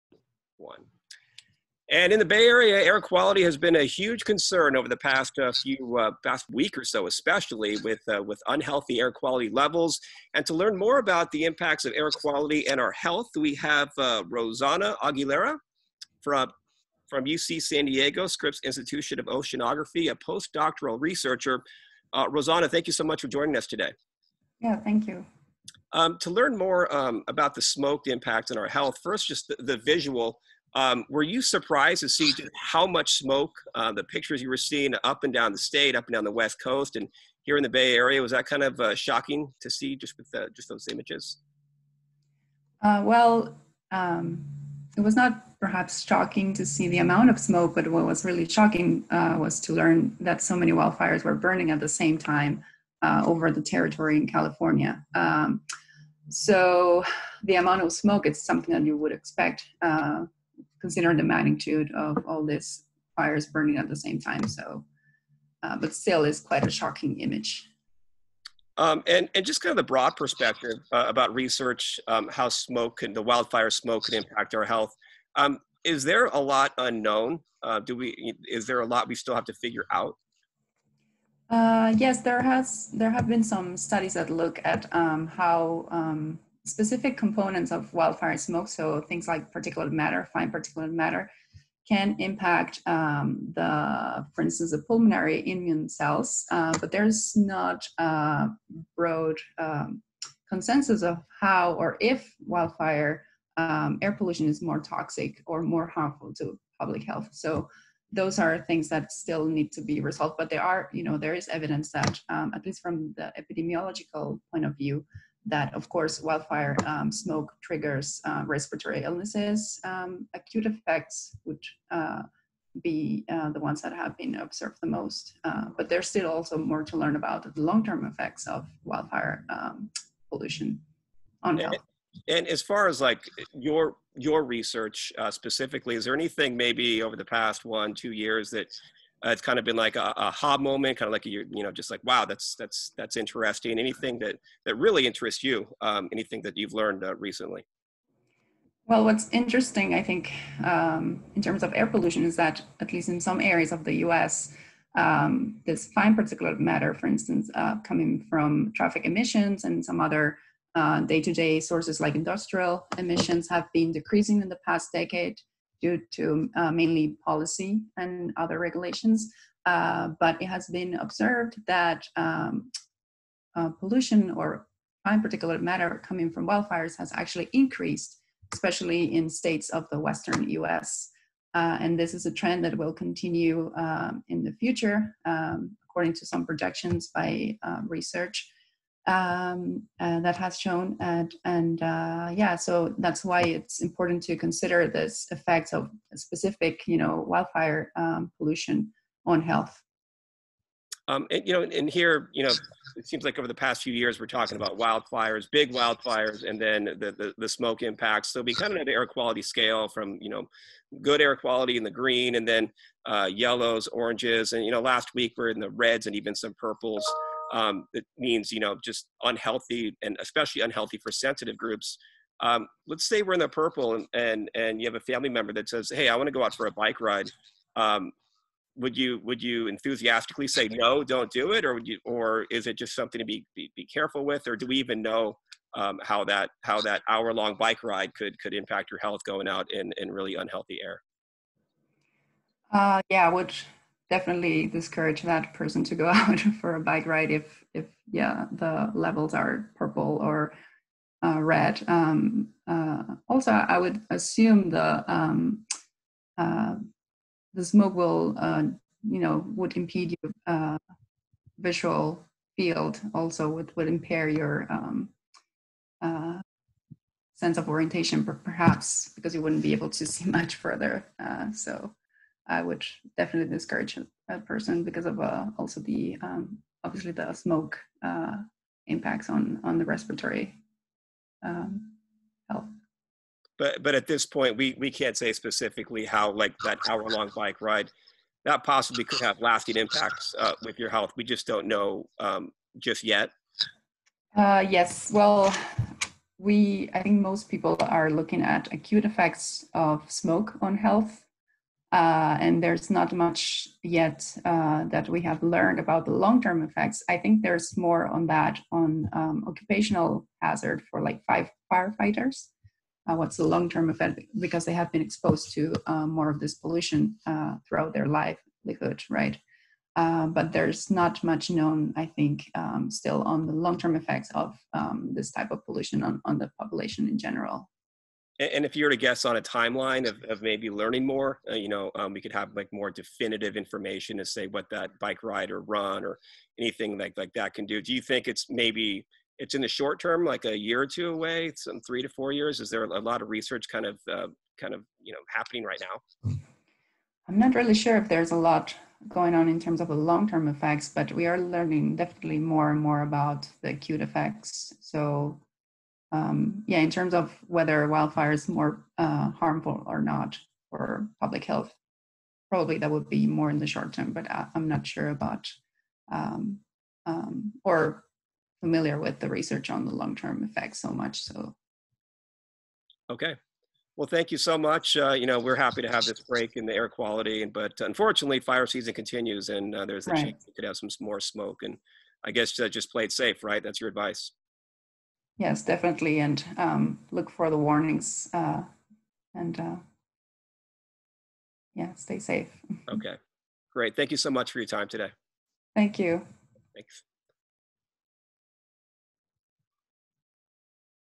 Lingering wildfire smoke could have long term health impacts
Following last Wednesday's intense smoke around the Bay Area, KTVU's Mark Tamayo spoke with Rosana Aguilera, a Postdoctoral Researcher from the Scripps Institution of Oceanography at UC San Diego. Aguilera has been researching wildfire smoke, and patterns of respiratory illness.